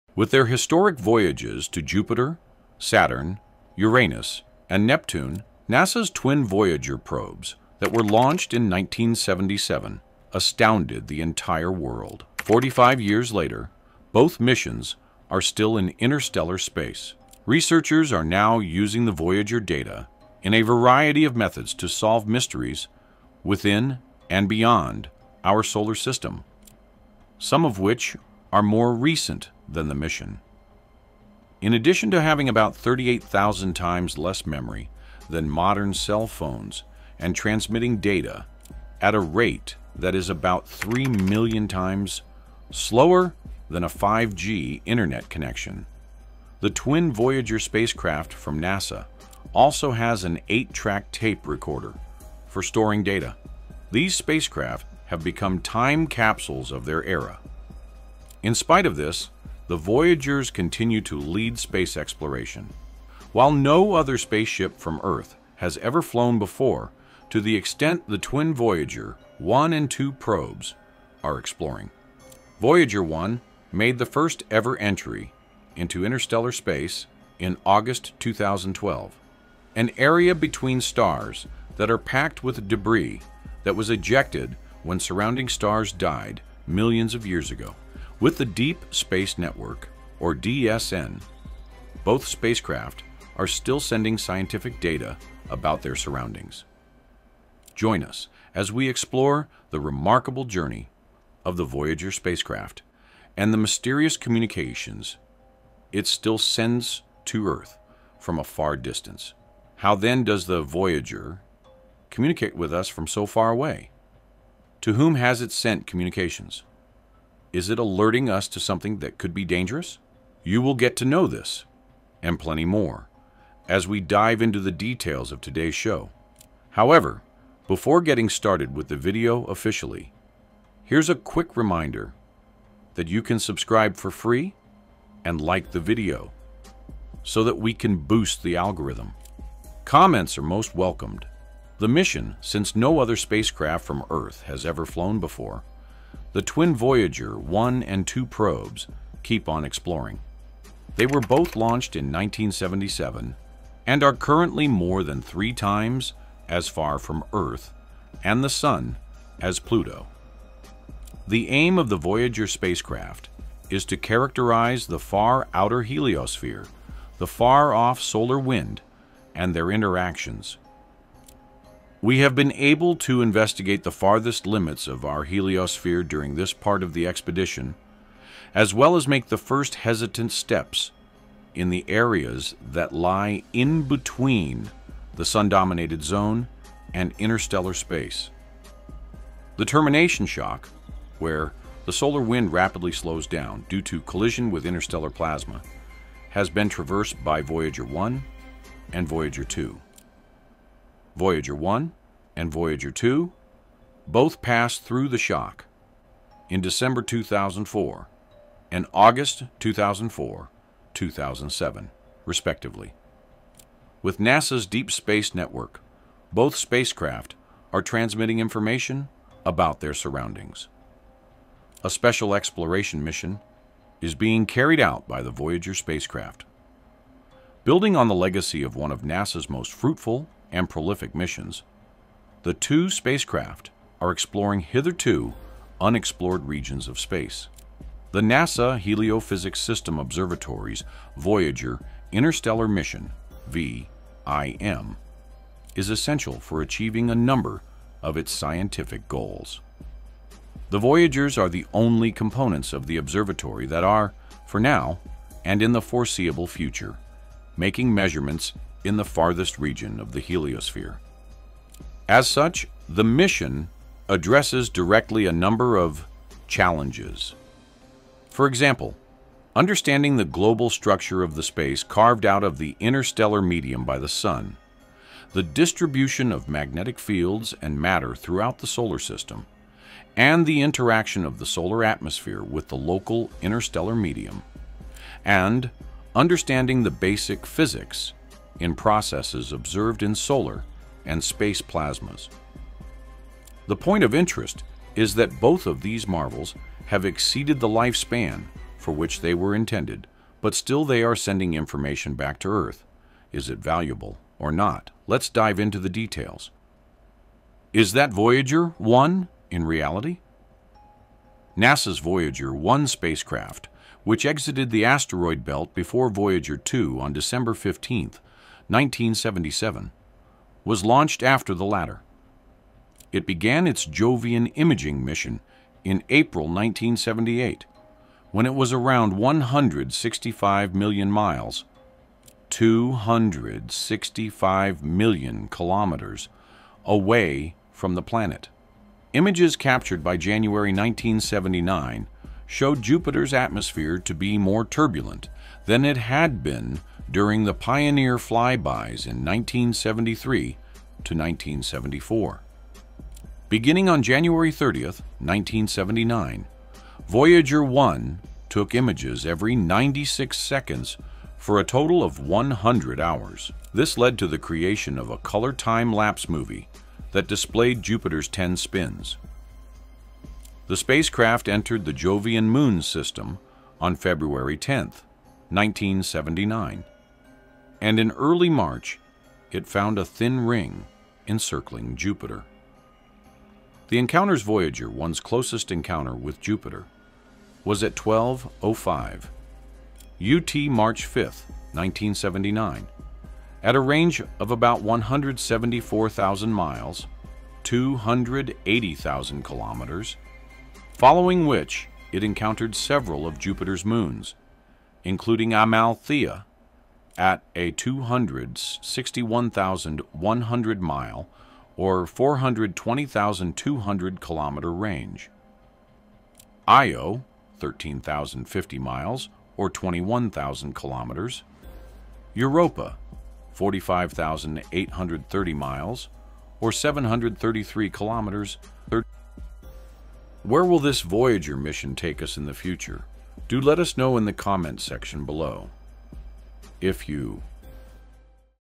VOYAGER 1 Sent Alien Message 45 Years Ago BUT Did They Reply?
Explore the mysteries of space as we delve into the fascinating story of Voyager 1, a spacecraft that has been hurtling through the cosmos for over four decades. In 1977, Voyager 1 sent a message into the unknown, a golden record containing sounds and images of humanity. But did we ever receive a response from any potential alien life form? Join us as we unravel the intriguing tale of Voyager 1's interstellar journey and the possibility of extraterrestrial contact. Is it possible that we're not alone in the universe? Let's find out!

Interstellar Shadows Presents VOYAGER 1 Sent Alien Message 45 Years Ago BUT Did They Reply?